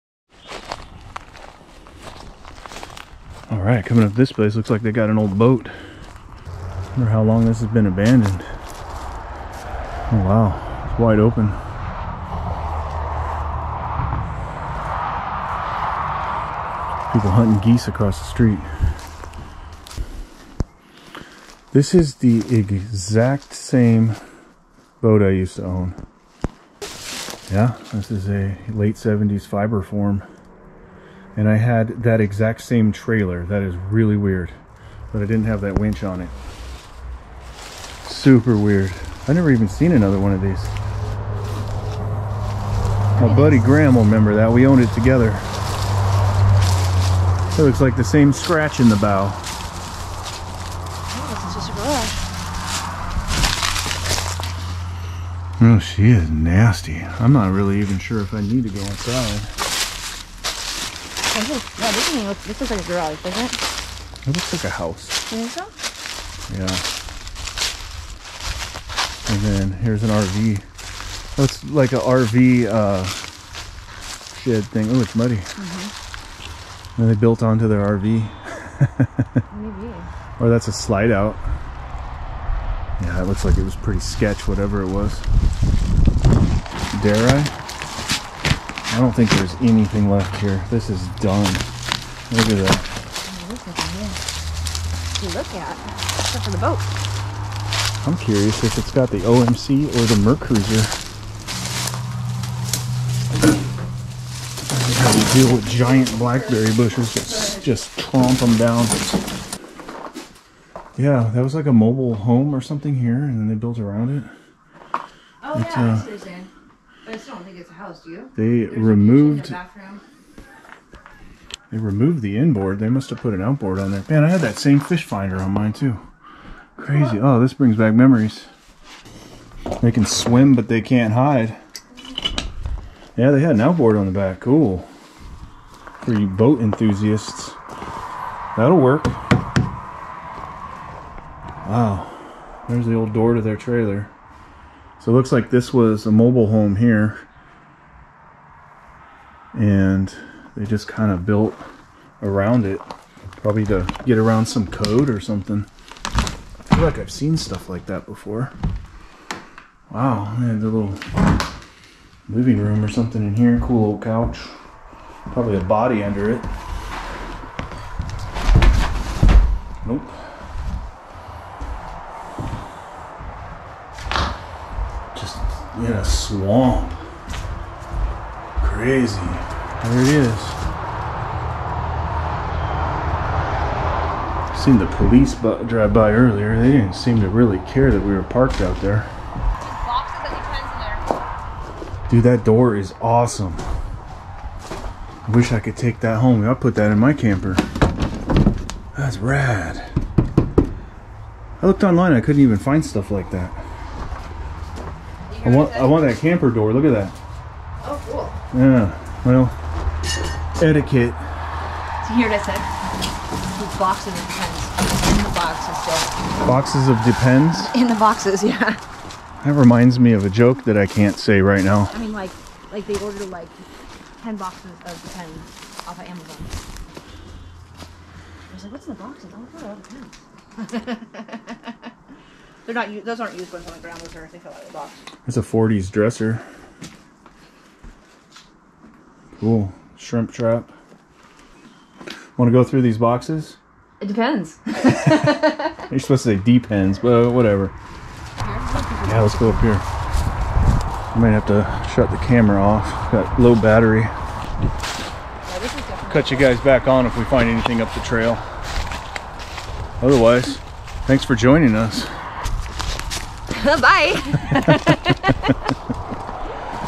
Can You Guess What They Left Behind?...Depends | WA HWY 101 abandoned house
Exploring an abandoned house on HWY 101 in Washington State, another urbex roadside attraction! Appears to be a trailer home with sketchy additions in a state of extreme decay. Late 1970's Fiberform boat with a nice Ez-loader trailer left behind along with boxes of Depends. Looks to have been abandoned in the last 10 years.